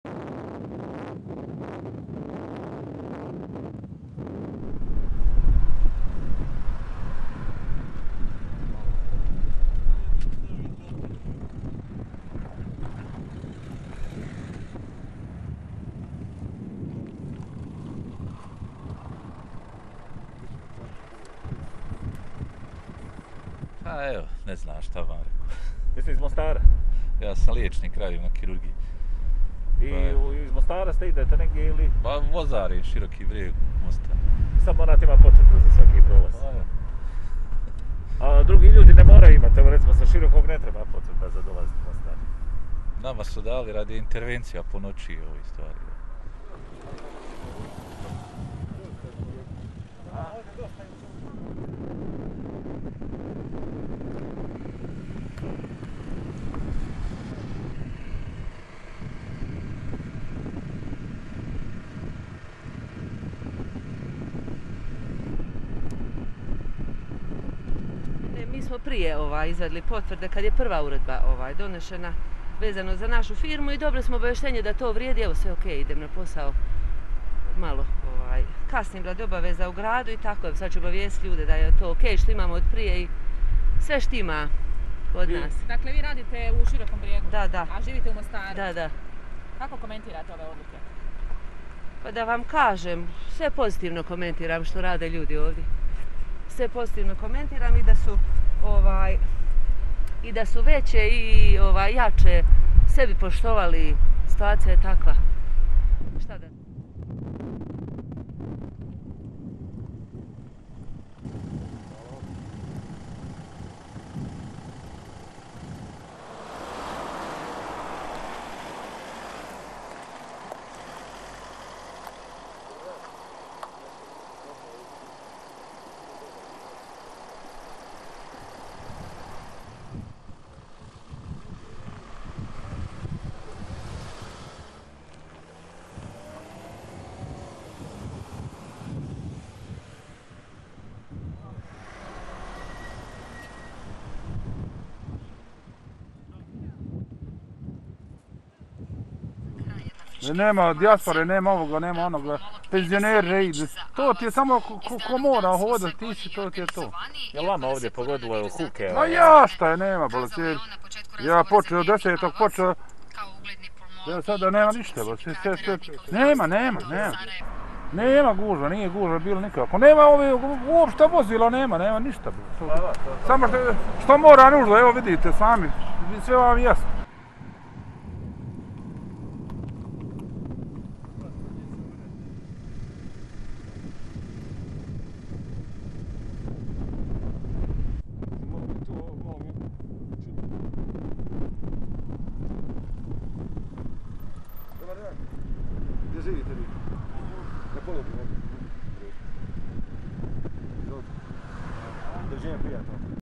Zabrana kretanja za stanovnike HN kantona - "Umjesto sprida možda se ponovo može ući sazad".
Stanovnici Hercegovačko-neretvanskog kantona u BiH svoja mjesta prebivališta mogu da napuštaju samo uz posebna odobrenja.